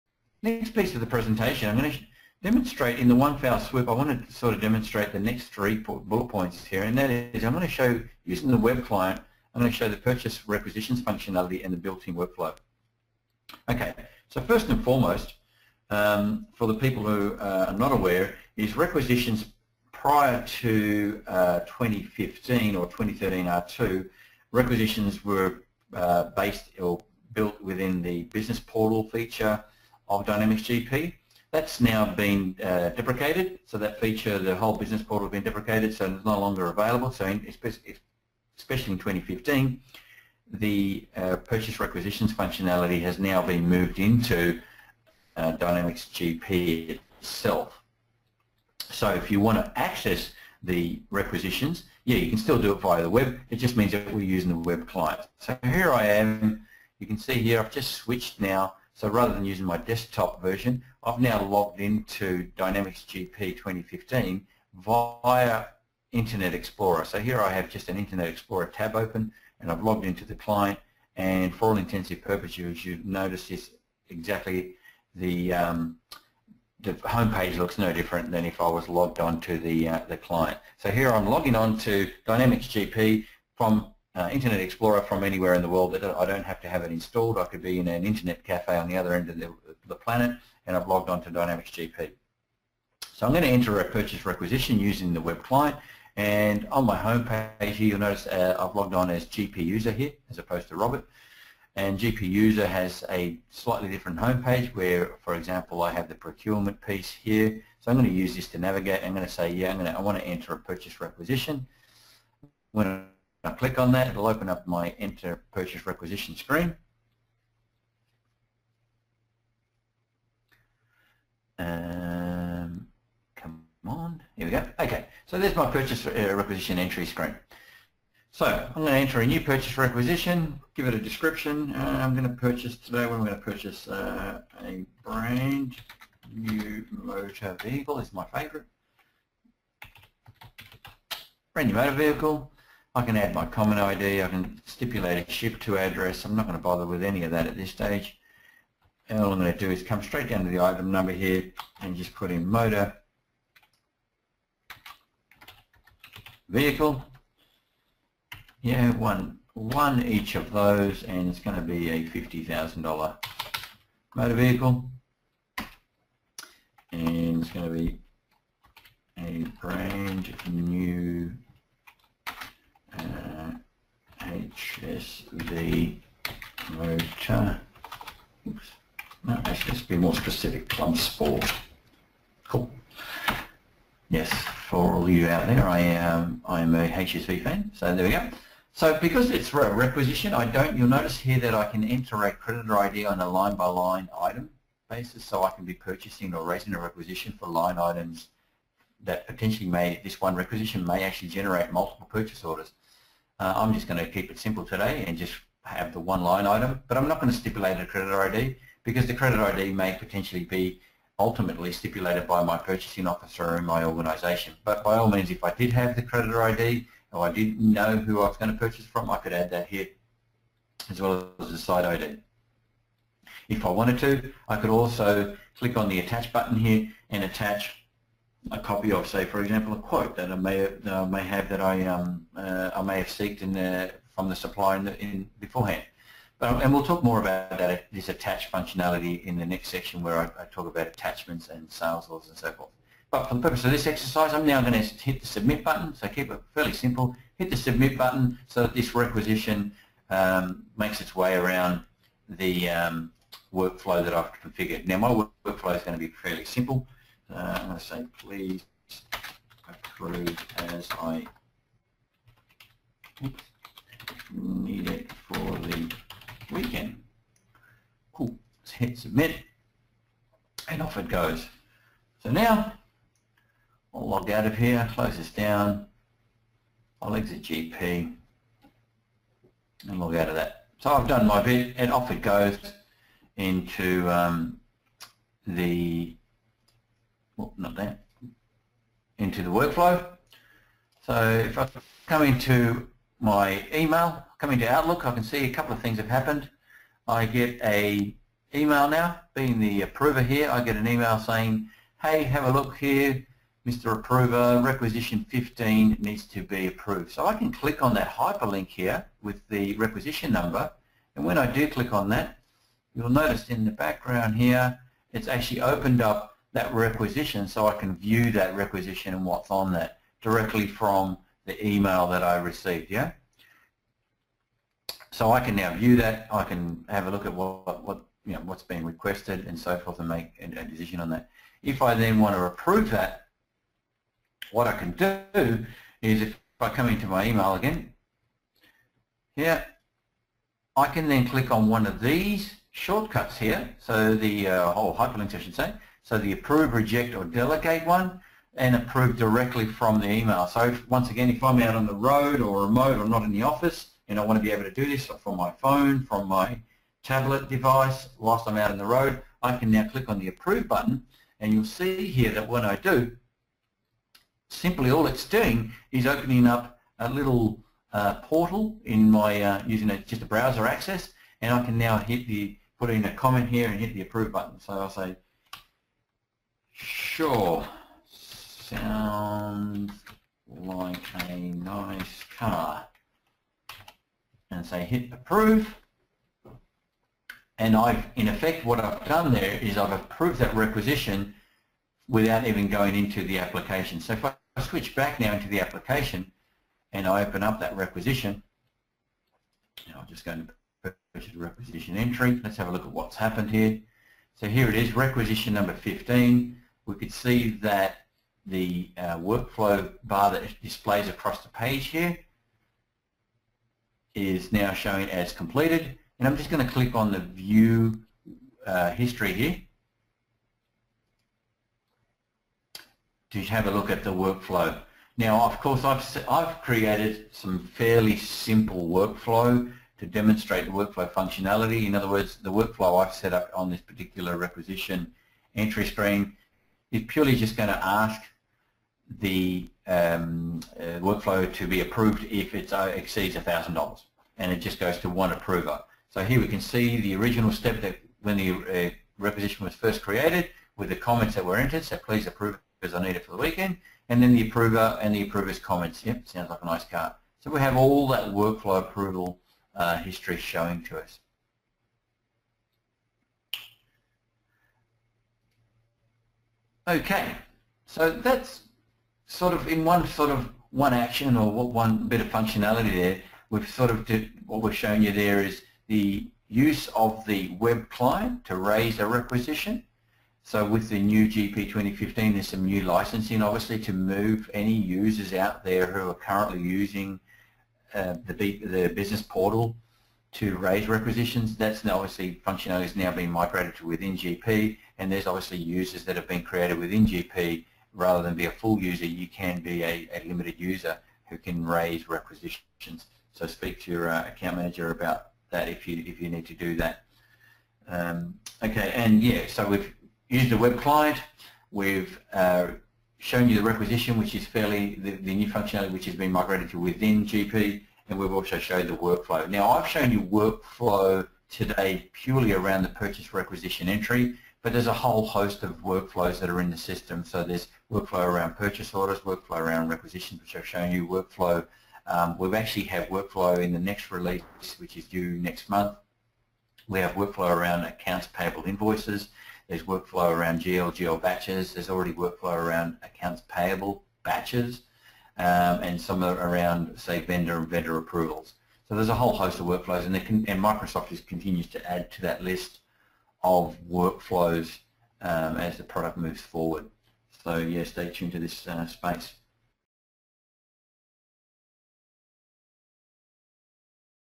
Microsoft Dynamics GP 2015 New Features - Web client, Purchase requisitions and built in workflows
Speak to Australia's largest Dynamics GP partner about upgrading your Dynamics GP.